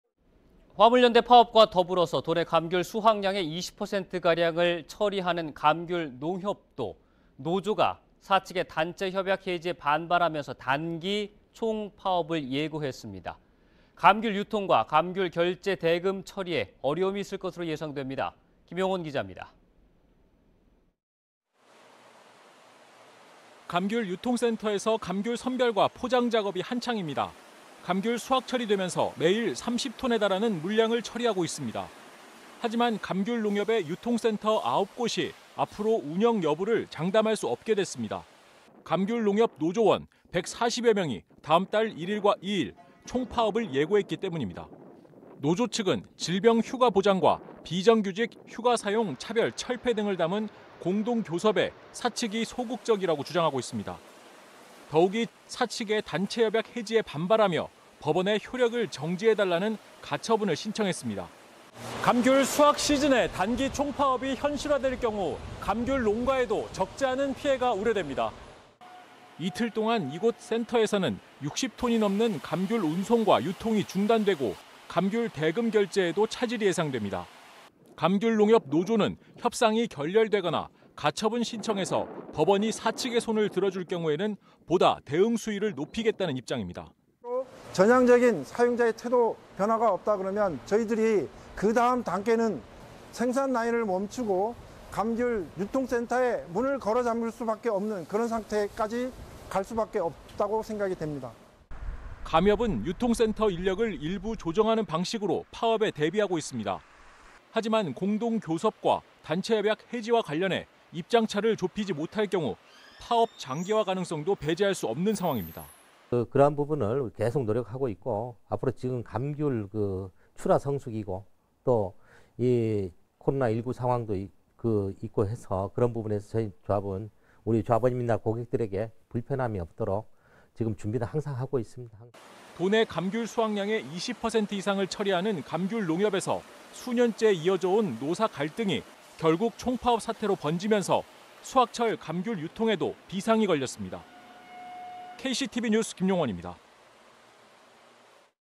감귤농협도 총파업?…수확철 유통 '비상'
화물연대 파업과 더불어 도내 감귤 수확량의 20% 가량을 처리하는 감귤농협도 노조가 사측의 단체 협약 해지에 반발하며 단기 총파업을 예고했습니다.

감귤 유통과 감귤 결제대금 처리에 어려움이 예상됩니다.